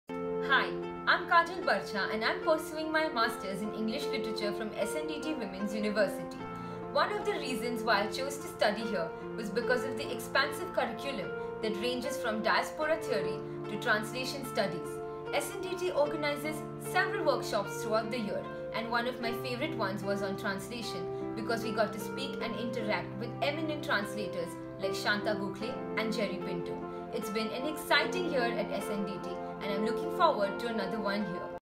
Student Testimonial Video 4
Here is what our students have to say about their experience at the Department. Have a look at our series of student testimonials.